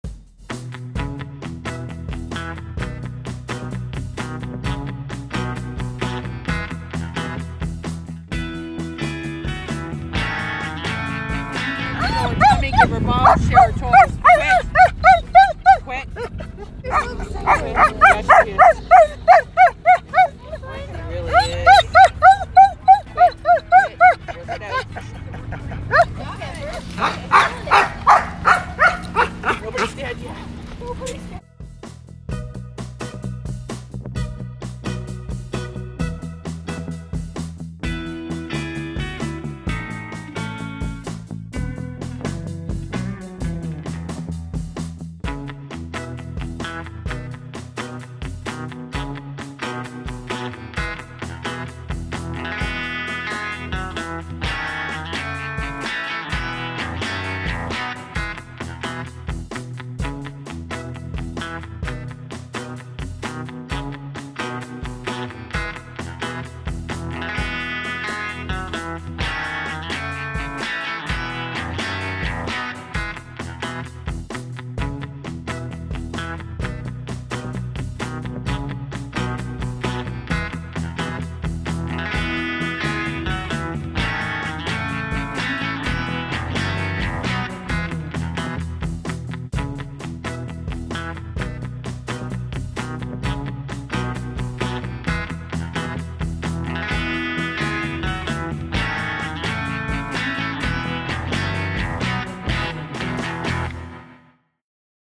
Reactive German Shepard Trains On E-Collar
Hello “Embrr”! 🤩
This beautiful 2 year old GSD came to our board and train program for dog reactivity, resource guarding, and general obedience. 🔥
This smarty pants was a hyper lady who was easily over excited!
But after her board and train she learned perfect heel work, place command, stay, recall and all this around other dogs. 🐶
Great work, Embrr! 💫